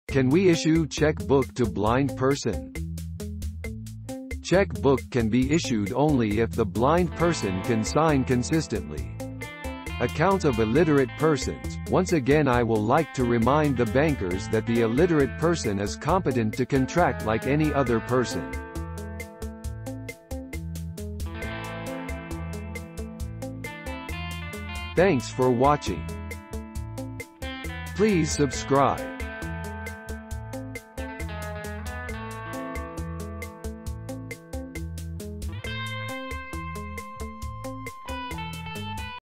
Can we issue Cheque book to blind person?
Accounts of Blind Persons, Illiterate Persons and Pardanashin Women

Cheque book can be issued only if the blind person can sign consistently. Accounts of illiterate persons : Once again I will like to remind the bankers that the illiterate person is competent to contract like any other person.